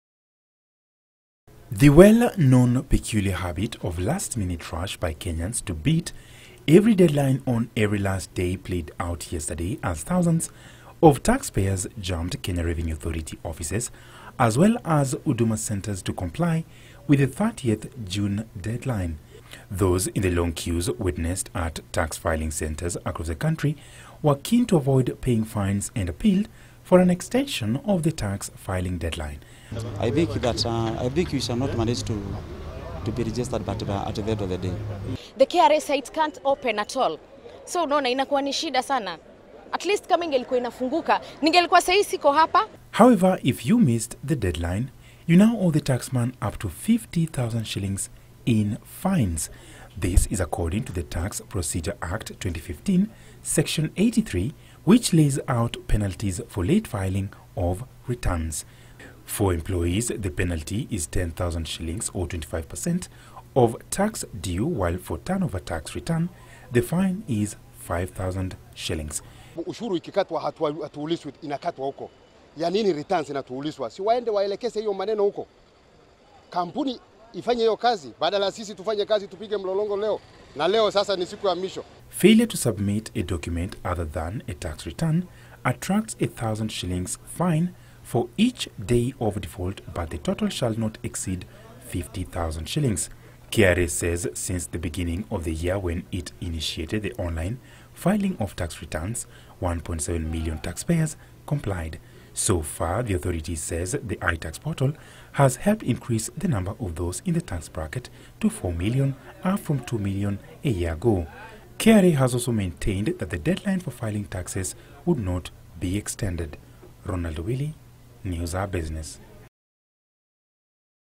Fines for iTax deadline defaulters
Did you file your tax returns with the Kenya Revenue Authority before yesterday’s deadline? If you did not, then be prepared to pay a fine of up to 50,000 shillings as the taxman reiterates that the deadline will not be extended. As of midnight, 1.7 million taxpayers had filed their tax returns through the online portal, iTax.